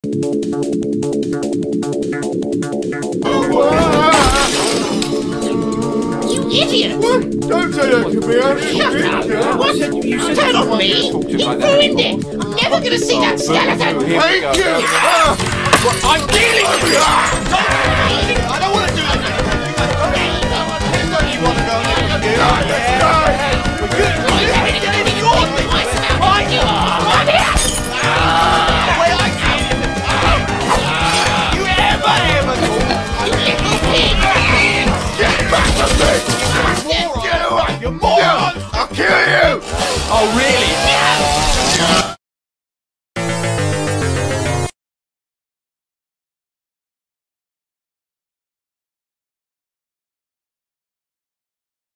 Ralphie, Wanda, and Jyoti Fighting
Talking Gallahad Minisode 18:
Fighting Over Broken Stuff

Outdate: September 5 2015